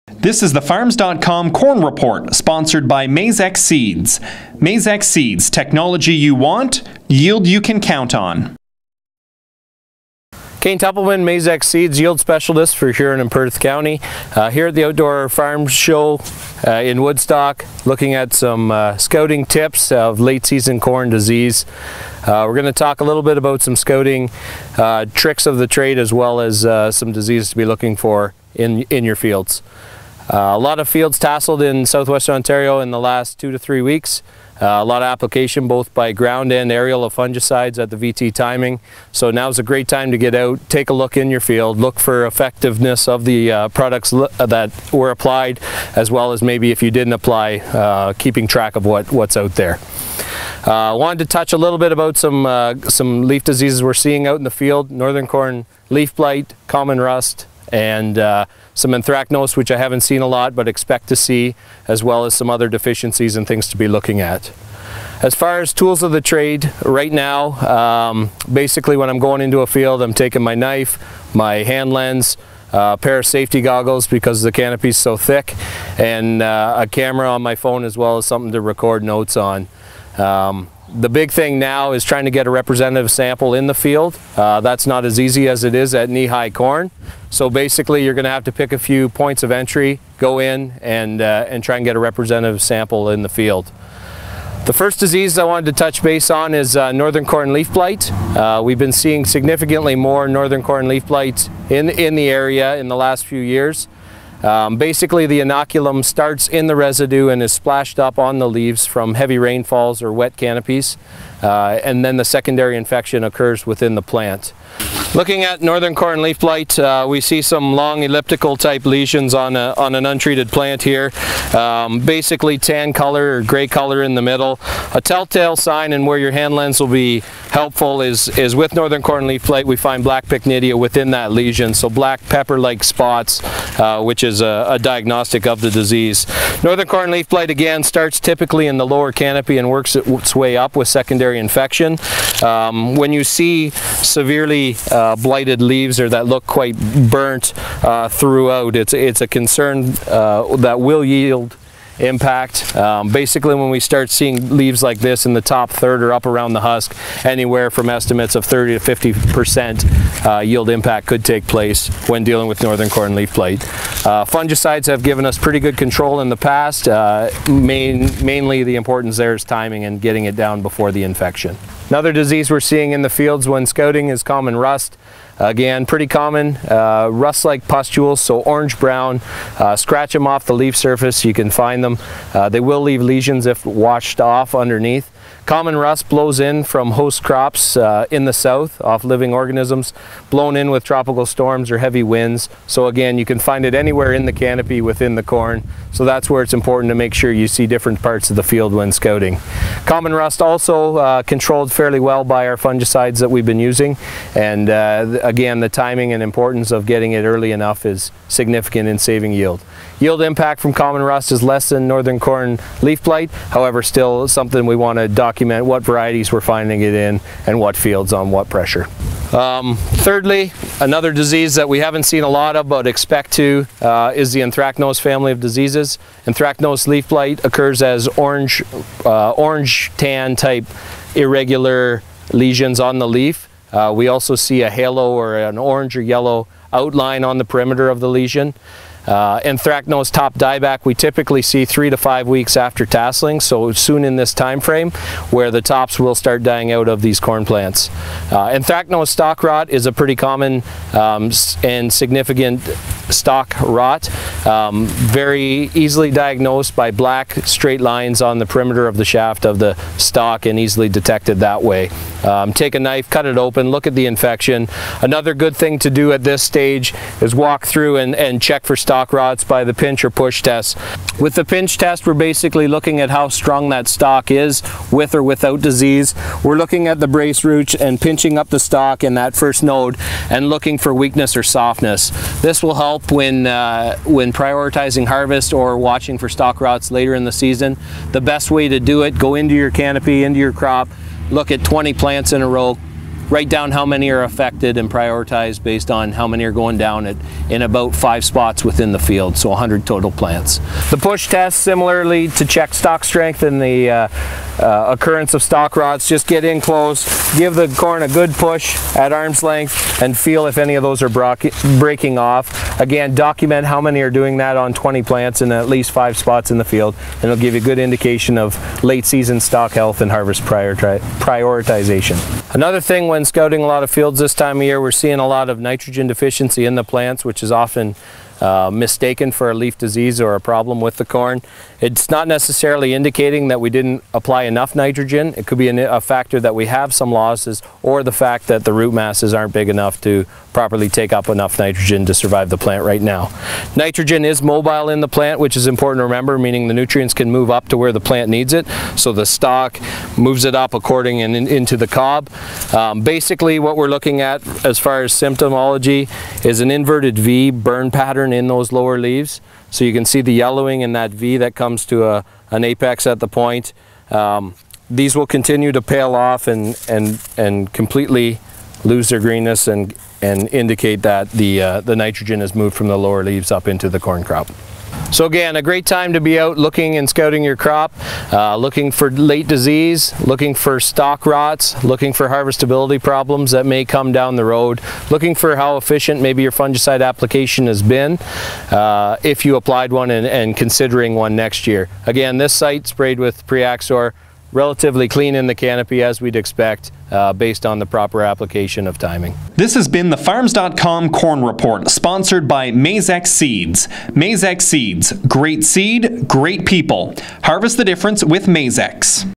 Farms.com Corn Report: Late Season Corn Disease Scouting Tips.
Agronomist Cain Templeman provides some tips on what farmers should be looking for in their fields at this stage of the corn crop.
Identifying disease pressure in the corn crop will indicate how will the fungicides have performed or if a fungicide program is needed next year.